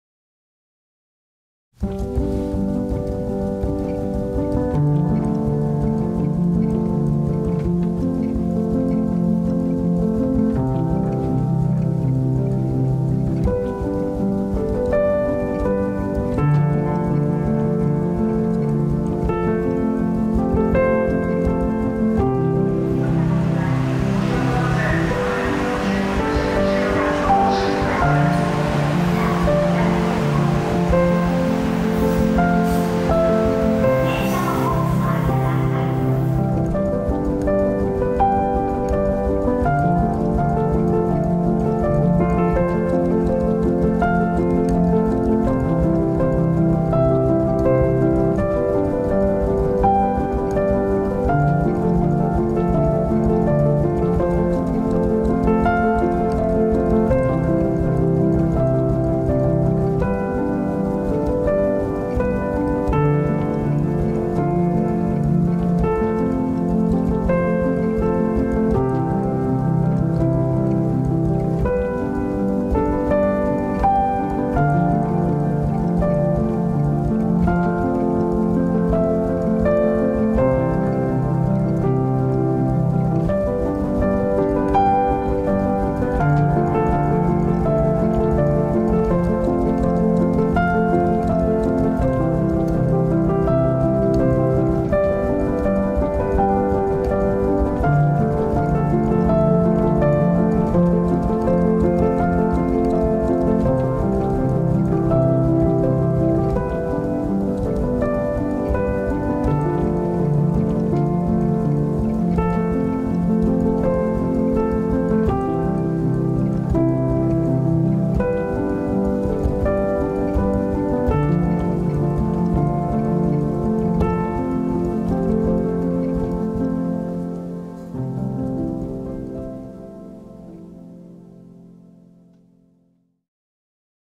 Nagoya station 【SONYα7s III/SEL135F18GM/DJIRS2】
SEL135F18GMレンズで一本で名古屋駅周辺の撮影に挑戦してみました。
パワーの有るジンバルRS2でも中望遠レンズでのジンバル撮影は
本当に難度が高かったです。
ブレが多いですが宜しければご覧下さい。